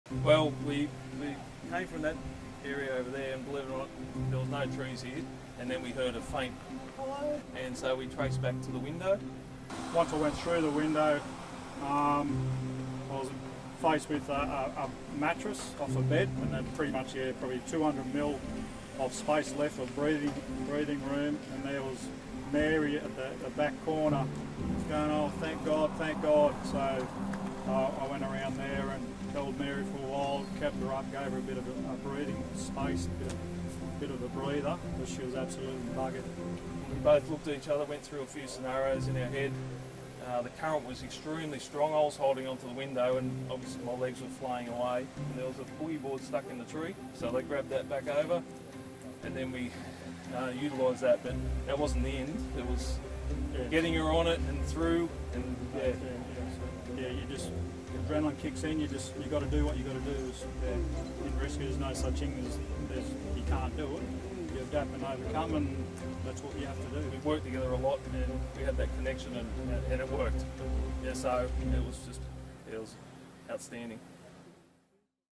Incredible rescue in Lismore floods - NSW Police Force
Earlier this week, Senior Constable Ben King and Senior Constable Adam Davis showed incredible bravery, along with two civilians, as they rescued 93-year-old Mary from her flooded home in Lismore.

The pair were able to manoeuvre Mary through a window using just a boogie board, moments before the house was fully submerged by the flood water.

While each of the countless rescues and stories of courage emerging from this disaster deserve recognition, yesterday Adam and Ben shared their story...